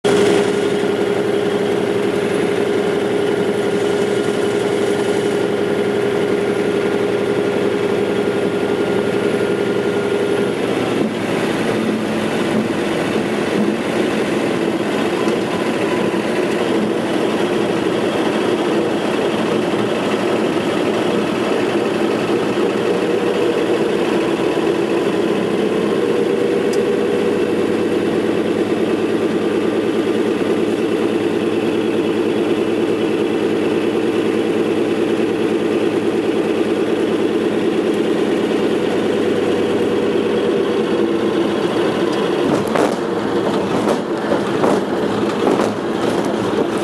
Antonov An-2
Landing with Antonov An -2, Chiaka Airport near Kiev, Ukraine...